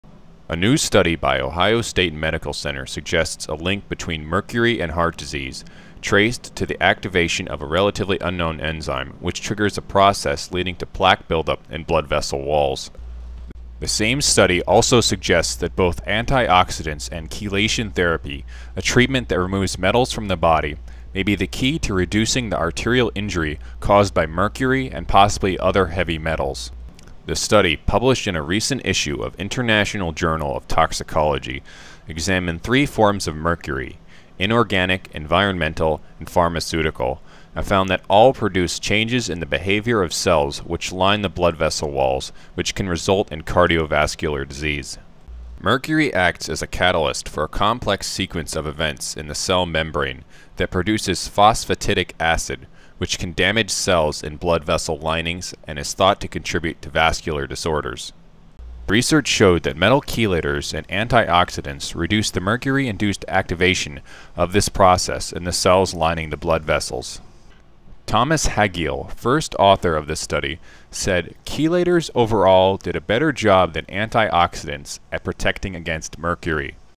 Empowered Health News | Chelation Therapy
A new study suggest positive effects of chelation therapy.

A new study by Ohio State Medical Center suggests a link between mercury and heart disease, traced to the activation of a relatively unknown enzyme, which triggers a process leading to plaque buildup in blood vessel walls. The same study also suggests that both antioxidants and chelation therapy, a treatment that removes metals from the body, may be the key to reducing the arterial injury caused by mercury, and possibly other heavy metals.

Story is produced and provided by Empowered Medical Media, LLC
to see the full story